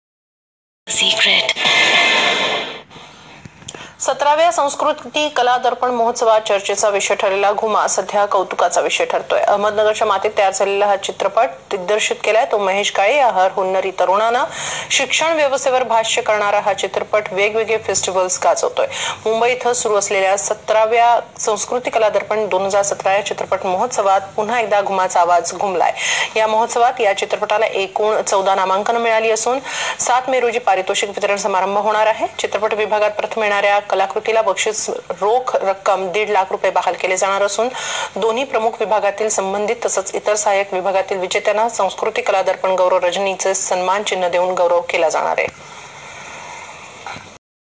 Ghuma Film Nomination News | Zee 24 Taas | 17th Sanskruti Kaladarpan
Ghuma Marathi Film won 14th Nominations in 17th Sanskruti Kaladarpan 2017.

After winning BEST VIEWRS CHOICE AWARD in PIFF.
it's new feather in crown of GHUMA.
FEELING PROUD TO BE PART OF THIS WONDERFUL PROJECT.
CONGRATES TEAM GHUMA AND THNX A LOT GHUMA TEAM